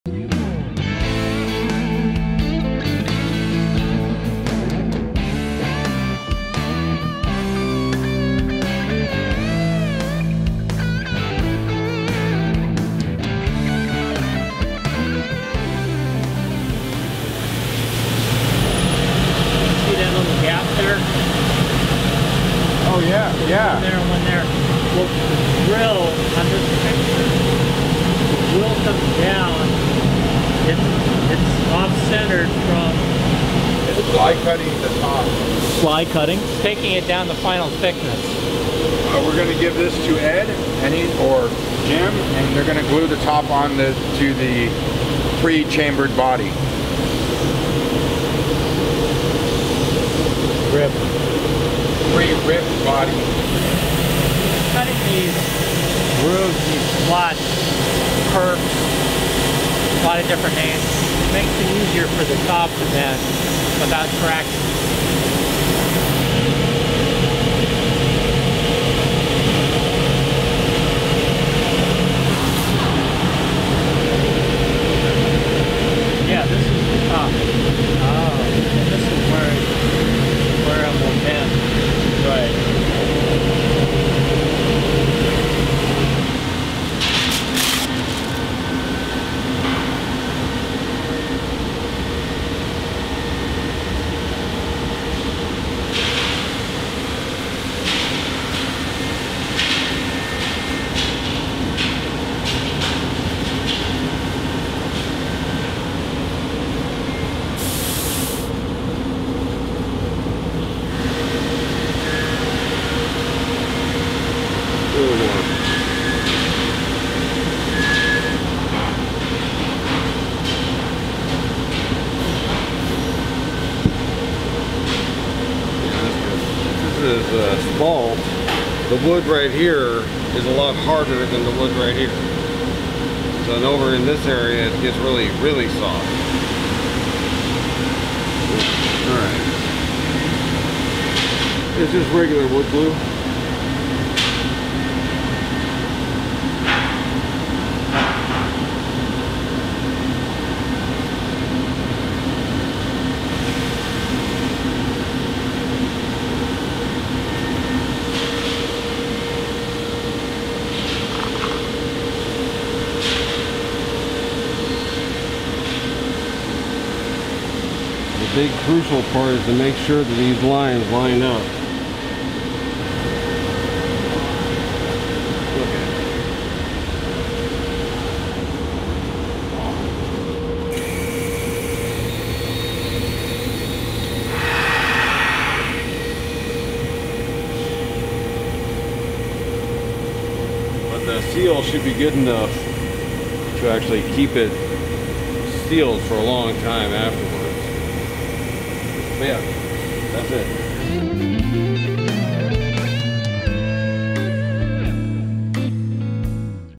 Watch James Tyler Working on the CNC Machine | James Tyler Guitars Factory
Here’s a video of Jim and Gil cutting a top with the CNC machine 🤖 In the second half of the video, you’ll see our master builder Ed gluing that same top to a “pre-ribbed body.”